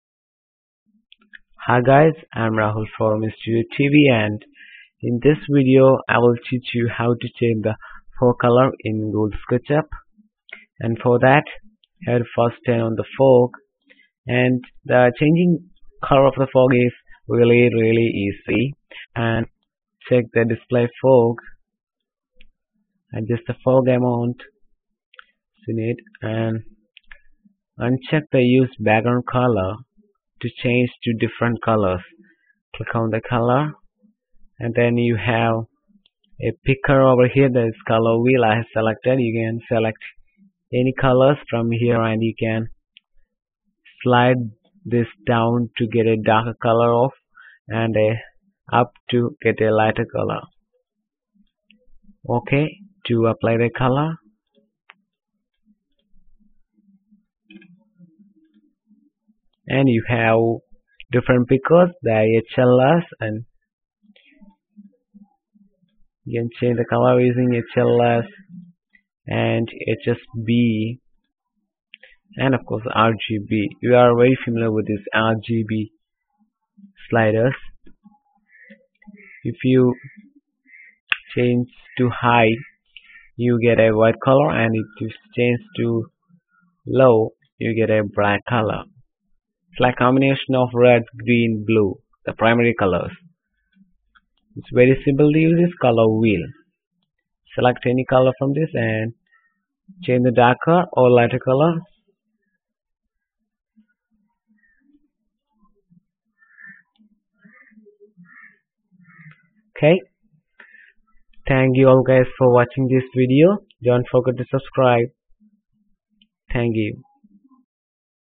How to change the color of Fog in Sketchup
Hey guys this video show you How to change the color of Fog in google sketchup